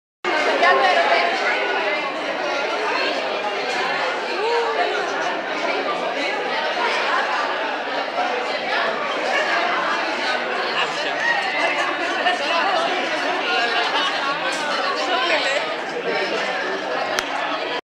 enikos.gr -NOMIKH SXOLH
Αναγνώστης του enikos.gr, φοιτητής στη Νομική Σχολή Αθηνών, έστειλε ένα βίντεο που «αποδεικνύει το χάος του Ελληνικού πανεπιστημίου», όπως σημειώνει χαρακτηριστικά.